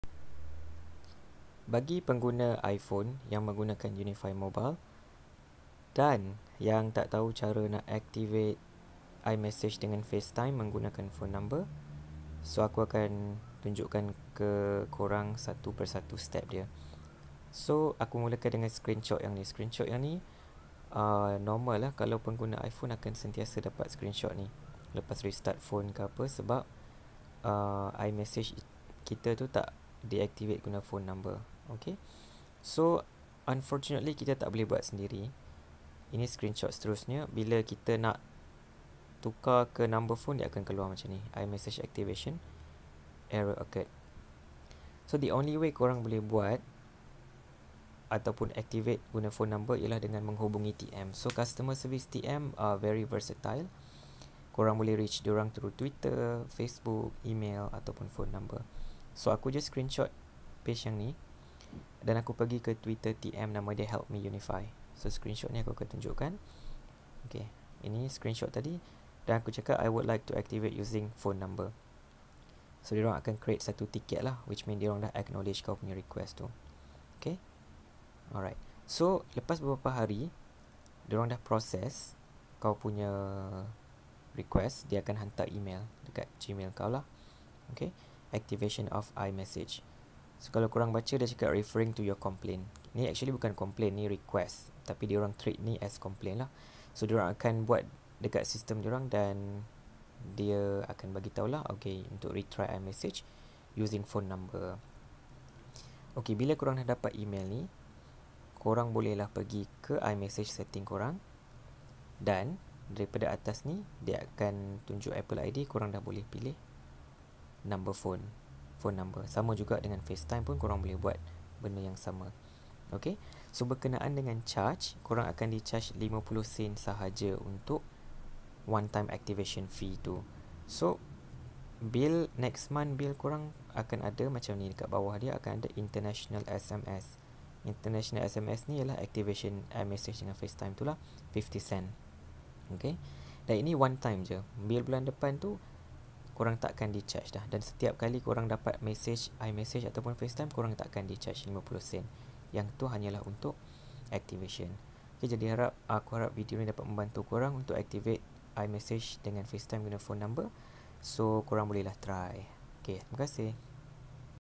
Unifi Mobile iMessage dan FaceTime menggunakan nombor telefon
Aktifkan iMessage dan FaceTime menggunakan nombor telefon dan bukan Apple ID bagi pengguna Unifi Mobile.